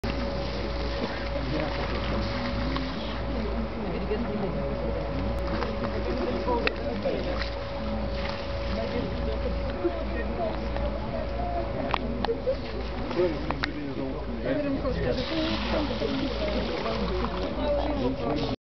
Plitvice National Park, Croatia
When you take the little boat ride across one of the lakes, these charming ducks fly after you expecting food..